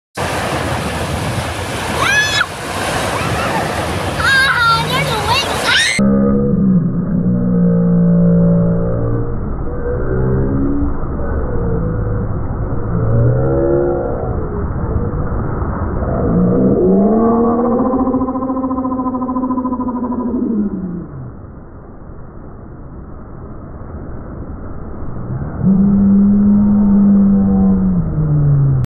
Slow Motion Wave Smacks Child
Ariel Hit right after saying, Ha Ha no more Waves. Full Speed and Slow Motion. Hilarious!!
Early Summer 2013